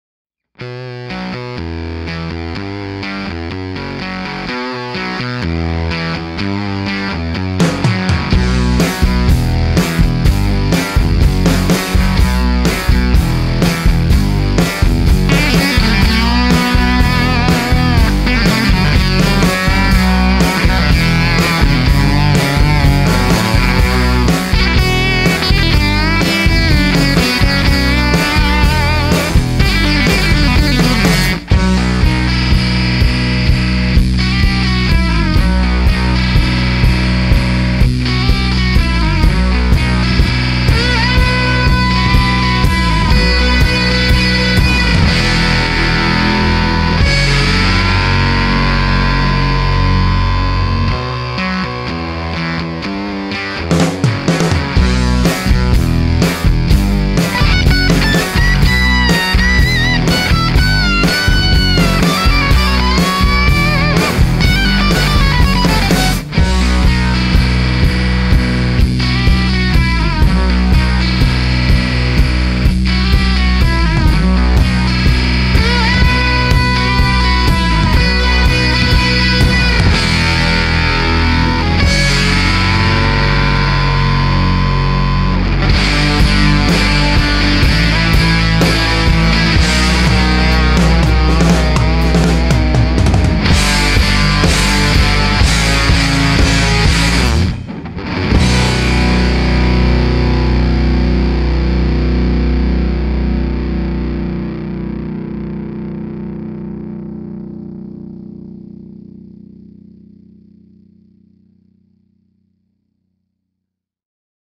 Be Cool Stay Cool Hudson Electronics UK Adverts
Music from my pedal demo of the Hudson Electronics Standard Machine with archive film footage of London streets and adverts. The "Standard Machine" is a sonic replica of the vintage Ibanez Standard Fuzz and the Shin-ei Superfuzz.